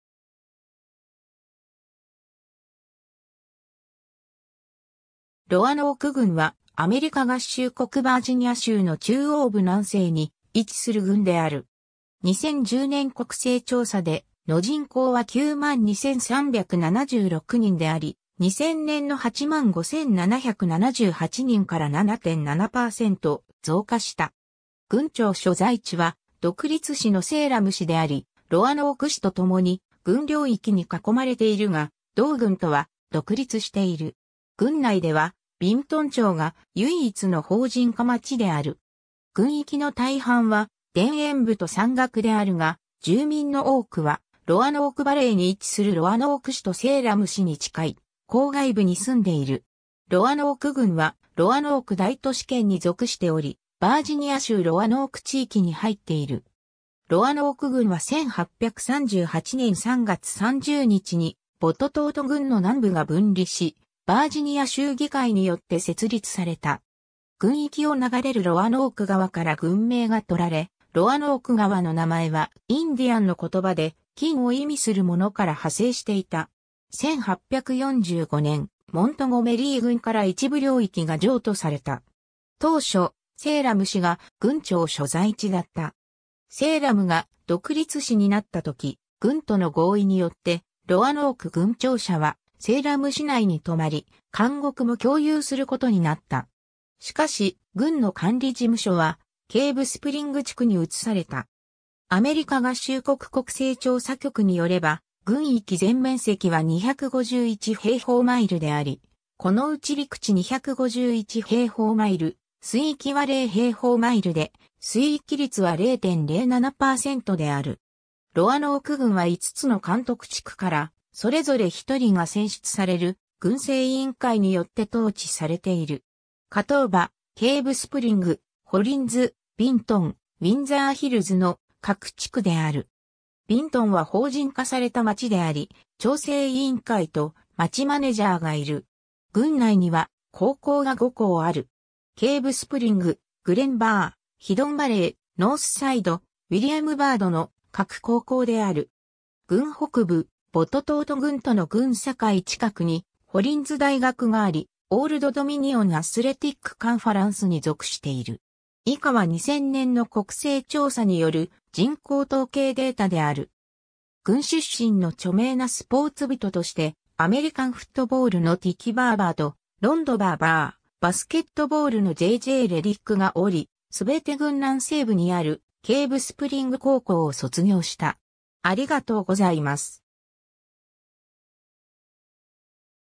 ロアノーク郡 (バージニア州)
ロアノーク郡（英: Roanoke County）は、アメリカ合衆国バージニア州の中央部南西に位置する郡である。
2010年国勢調査での人口は92,376人であり、2000年の85,778人から7.7%増加した。
郡庁所在地は独立市のセイラム市（人口24,802人）であり、ロアノーク市と共に、郡領域に囲まれているが、同郡とは独立している。
郡内ではビントン町が唯一の法人化町である。
郡域の大半は田園部と山岳であるが、住民の多くはロアノーク・バレーに位置するロアノーク市とセイラム市に近い郊外部に住んでいる。
 ロアノーク郡はロアノーク大都市圏に属しており、バージニア州ロアノーク地域に入っている。
 ロアノーク郡は1838年3月30日に、ボトトート郡の南部が分離し、バージニア州議会によって設立された。
郡域を流れるロアノーク川から郡名が採られ、ロアノーク川の名前はインディアンの言葉で「金」を意味するものから派生していた。
1845年、モントゴメリー郡から一部領域が譲渡された。
当初、セイラム市が郡庁所在地だった。
セイラムが独立市になった時、郡との合意によってロアノーク郡庁舎はセイラム市内に留まり、監獄も共有することになった。
しかし、郡の管理事務所はケイブスプリング地区に移された。
 アメリカ合衆国国勢調査局に拠れば、郡域全面積は251平方マイル (650 km2)であり、このうち陸地251平方マイル (650 km2)、水域は0平方マイル (0 km2)で水域率は0.07%である。
 ロアノーク郡は5つの監督地区からそれぞれ1人が選出される郡政委員会によって統治されている。
カトーバ、ケイブスプリング、ホリンズ、ビントン、ウィンザーヒルズの各地区である。
ビントンは法人化された町であり、町政委員会と町マネジャーがいる。
 郡内には高校が5校ある。
ケイブスプリング、グレンバー、ヒドンバレー、ノースサイド、ウィリアム・バードの各高校である。
郡北部、ボトトート郡との郡境近くにホリンズ大学があり、オールド・ドミニオン・アスレティック・カンファランスに属している。
 郡出身の著名なスポーツ人として、アメリカンフットボールのティキ・バーバーとロンド・バーバー、バスケットボールのJ.J.レディックがおり、全て郡南西部にあるケイブスプリング高校を卒業した。